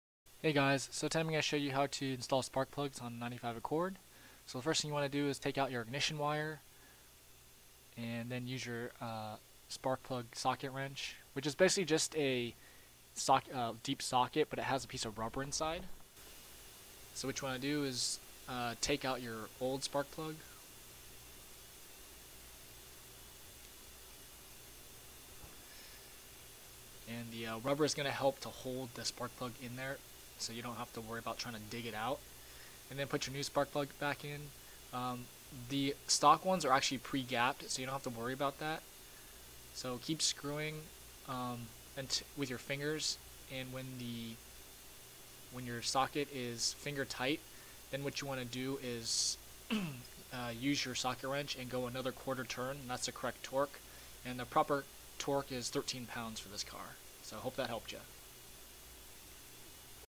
Tutorial: How to change Spark Plugs on a 1995 Honda Accord
Like cars?

Learn how to change spark plugs on a 1995 Honda Accord.  This tutorial will work on any car out there with an internal combustion engine.

Spark plugs on Amazon!